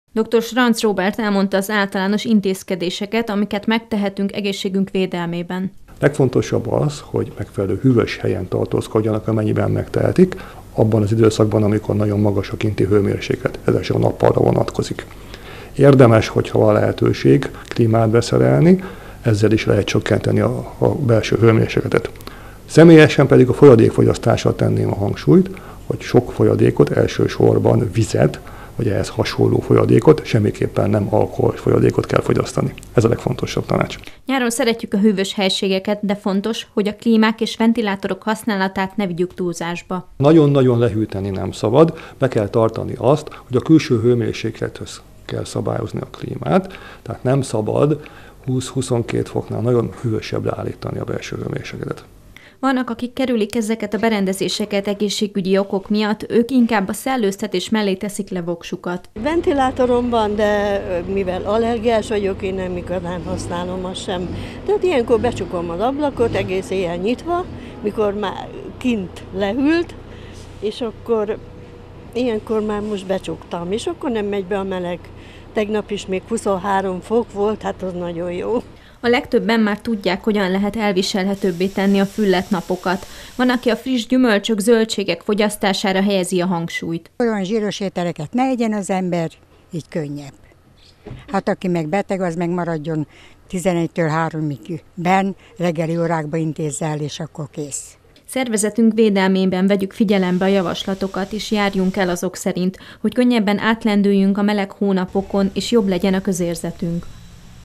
Hamarosan beköszönt a kánikula - hogyan készüljünk fel a melegre?
Hamarosan beköszönt a kánikula, nyitnak a strandok, előkerülnek a ventilátorok és egyéb hűtő berendezések. Helyieket kérdeztünk arról, hogyan készülnek fel, valamint hogy milyen praktikákat lehet bevetni a szervezetet igen megterhelő napokra.